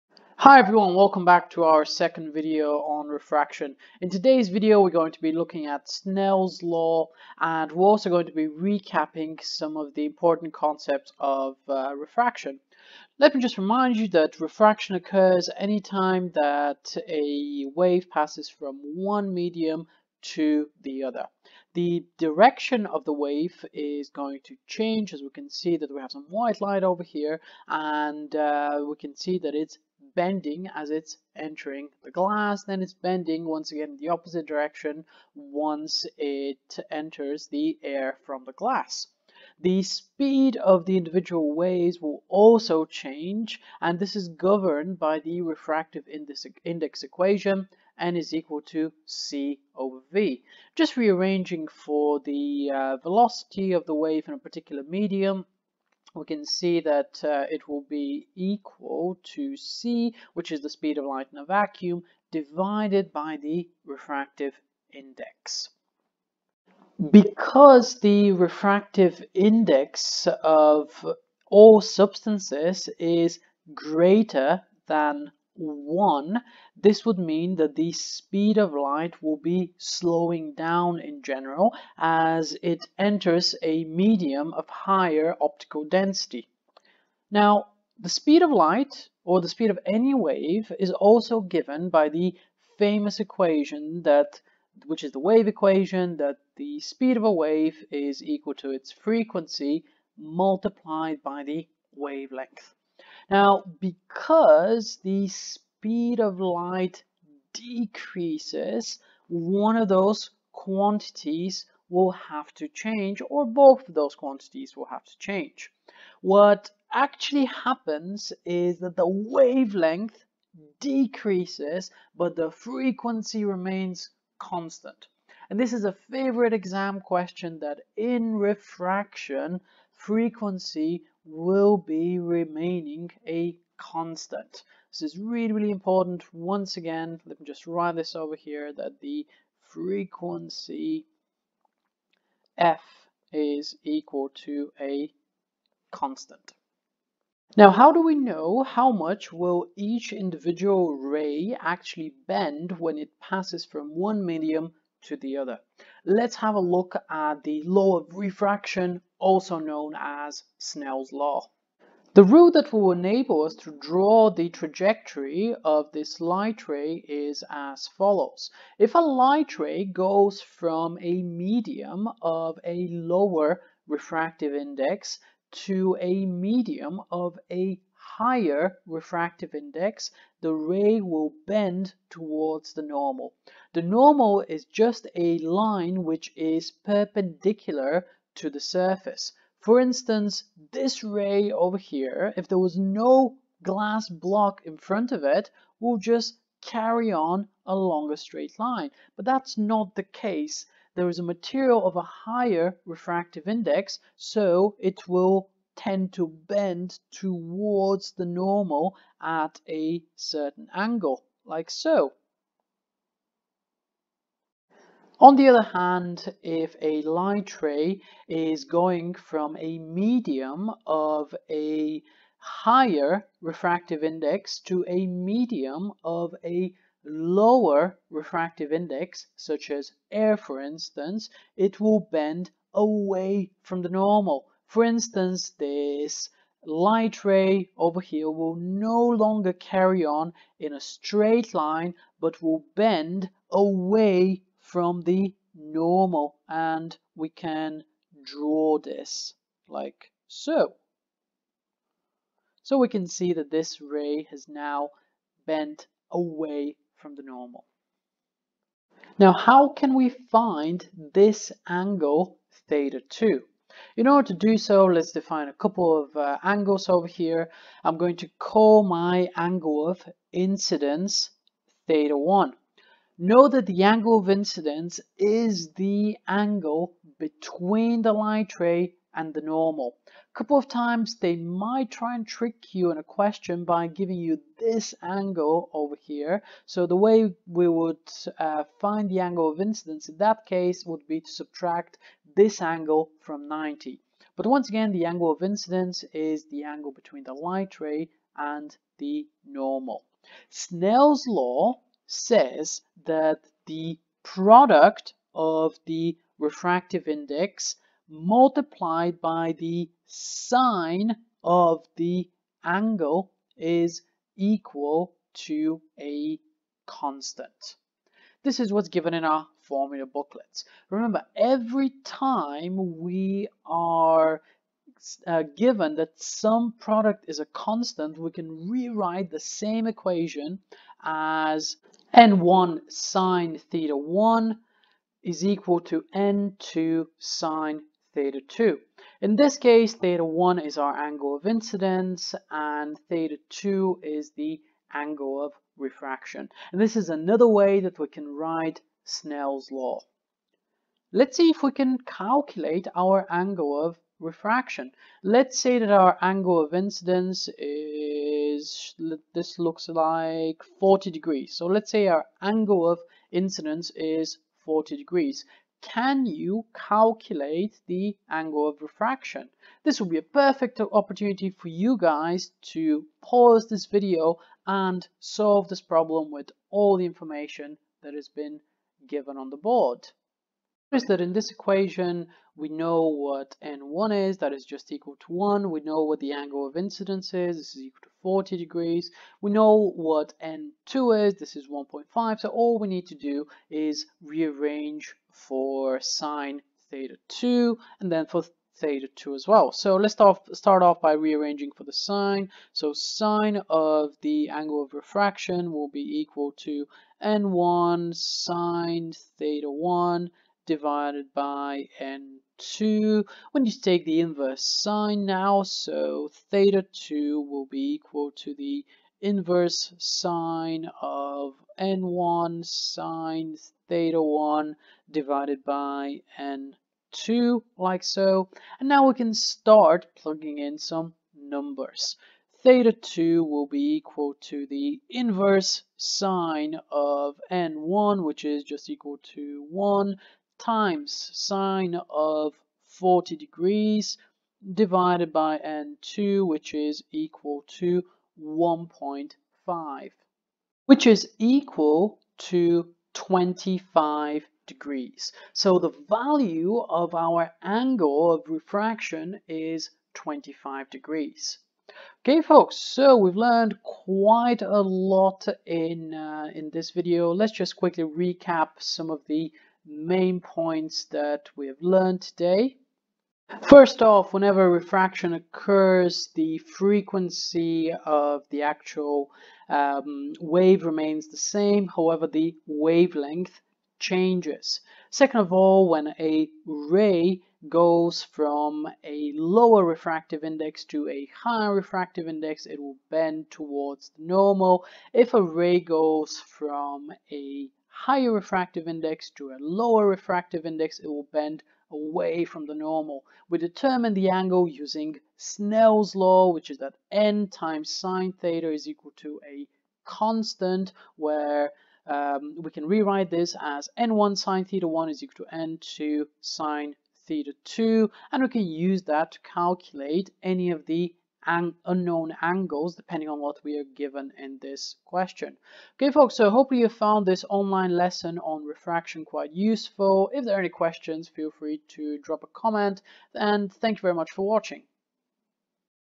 A Level Physics: Refraction and Snell's Law
Online lesson on refraction and Snell's law. Topics covered:
Chapters:
00:00 Intro and recap
01:07 Frequency remains constant
02:44 Refraction and the normal
05:00 Snell's Law
06:16 Example Problem and Solution